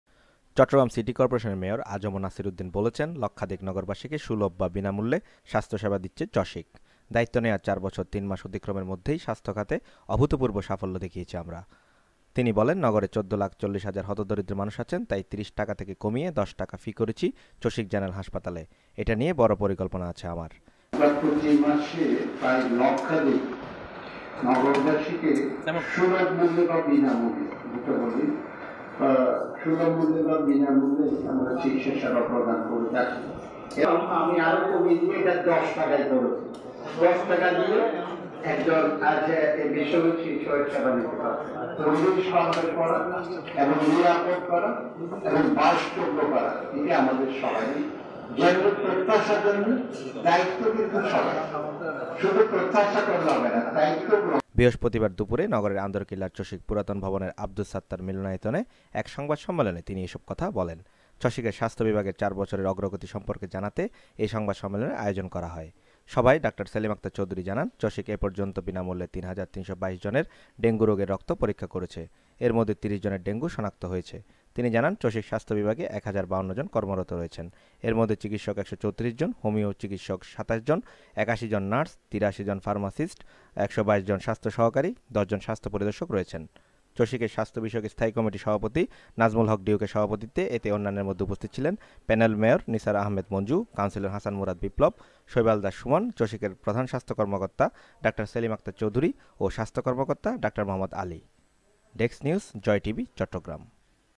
লক্ষাধিক নগরবাসীকে সুলভ মূল্যে স্বাস্থ্যসেবা দিচ্ছে চসিক
চট্টগ্রাম সিটি করপোরেশনের (চসিক) মেয়র আ জ ম নাছির উদ্দীন বলেছেন, লক্ষাধিক নগরবাসীকে সুলভ বা বিনামূল্যে স্বাস্থ্যসেবা দিচ্ছে চসিক। দায়িত্ব নেওয়ার ৪ বছর ৩ মাস অতিক্রমের মধ্যেই স্বাস্থ্যখাতে অভূতপূর্ব সাফল্য দেখিয়েছি আমরা।
তিনি বলেন, নগরে ১৪ লাখ ৪০ হাজার হতদরিদ্র মানুষ আছেন। তাই ৩০ টাকা থেকে কমিয়ে ১০ টাকা ফি করেছি চসিক জেনারেল হাসপাতালে। এটা নিয়ে বড় পরিকল্পনা আছে আমার।
বৃহস্পতিবার (১৭ অক্টোবর) দুপুরে নগরের আন্দরকিল্লার চসিক পুরাতন ভবনের আব্দুস ছত্তার মিলনায়তনে এক সংবাদ সম্মেলনে তিনি এসব কথা বলেন। চসিকের স্বাস্থ্য বিভাগের ৪ বছরের অগ্রগতি সম্পর্কে জানাতে এ সংবাদ সম্মেলনের আয়োজন করা হয়।